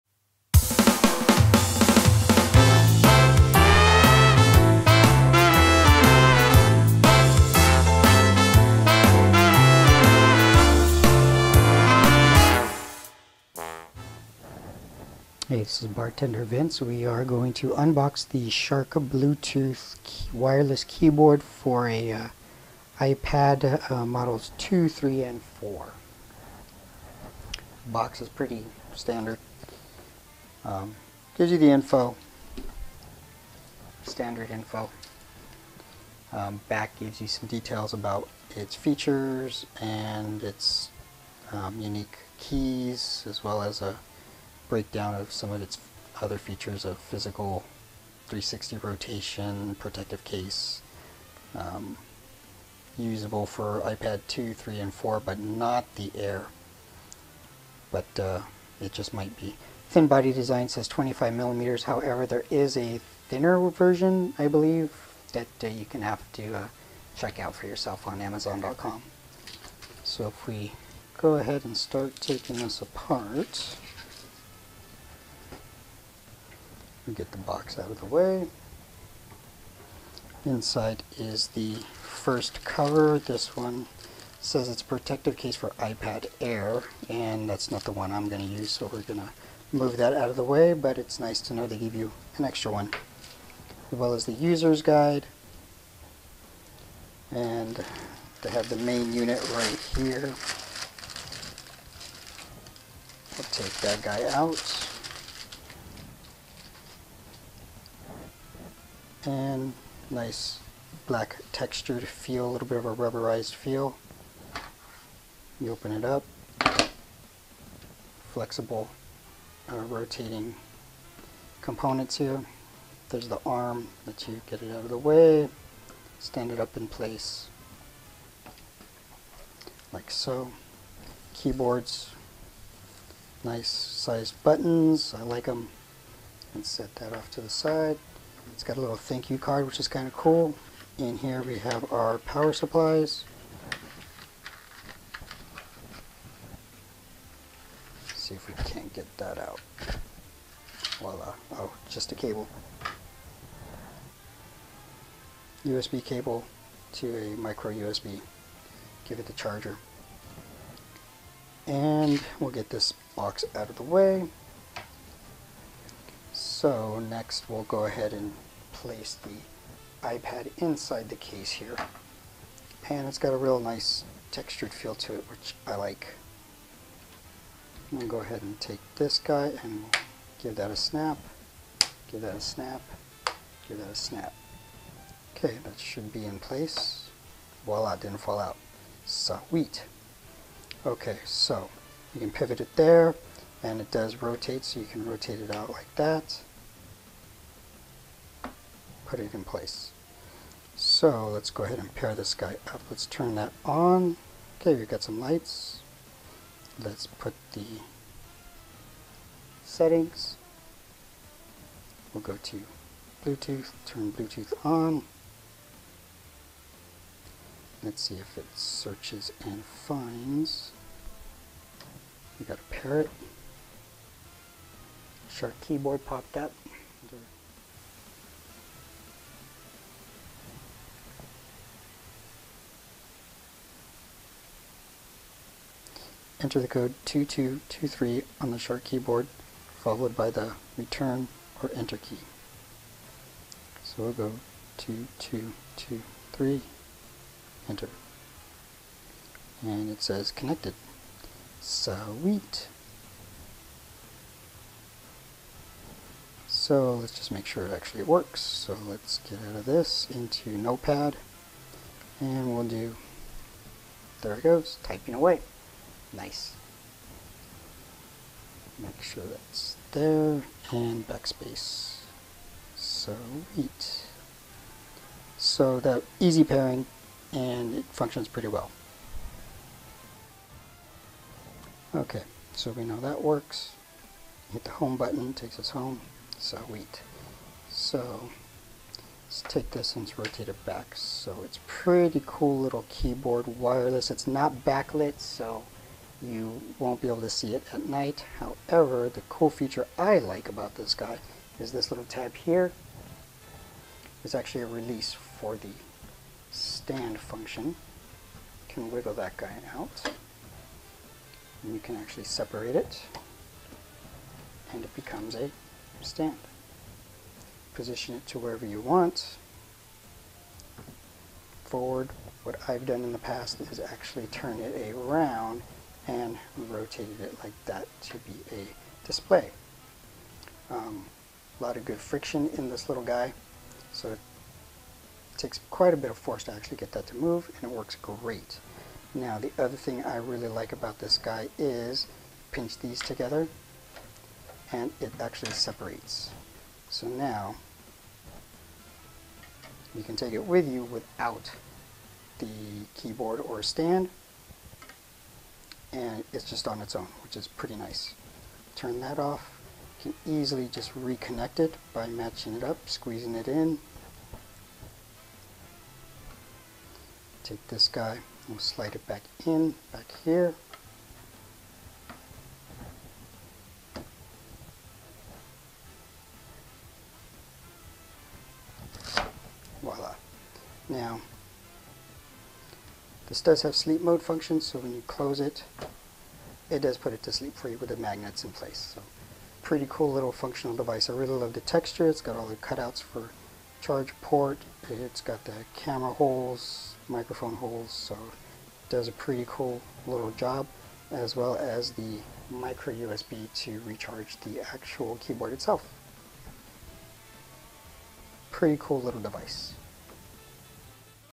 Unboxing Sharkk Bluetooth keyboard for iPad 2, 3, 4.
As a private bartender for hire, keep my business efficient and lean.  Here is one piece of equipment I use to be successful.

In this video I show you what's in the box, how to pair it and some cool points about this keyboard.